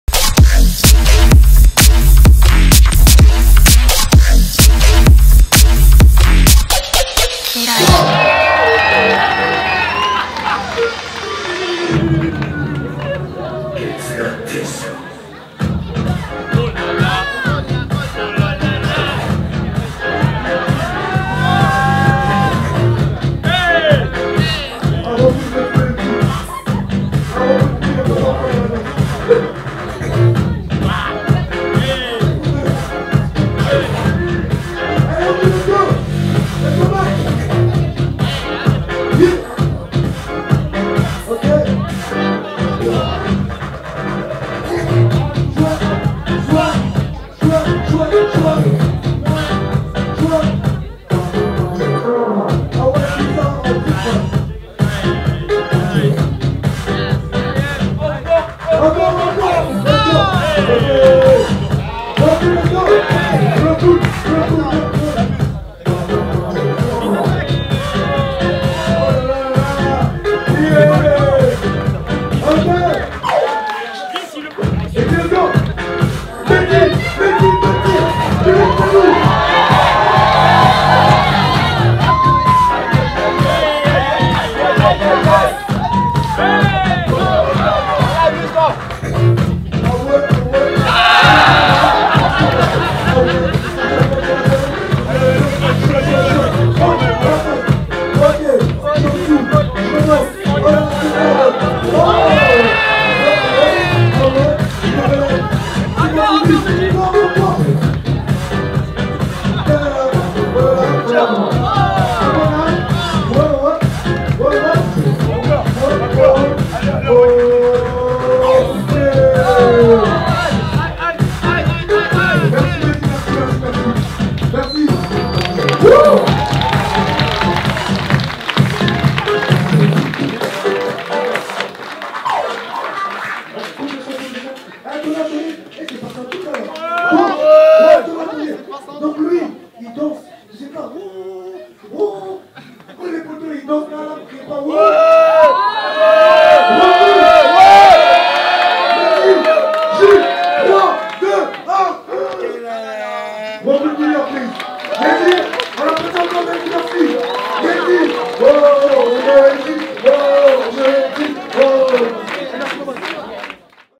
Spear 8 Groupe A10
BATTLE ELECTRO 3 en 1 - Festival 100% La Villette

JURY // Milliard (RK) - Glyslein Lefever (Blanca Li) - Skips (E.Street)
Jimmy Milliard Medina Glyslein Lefever Romain Skips Guillermic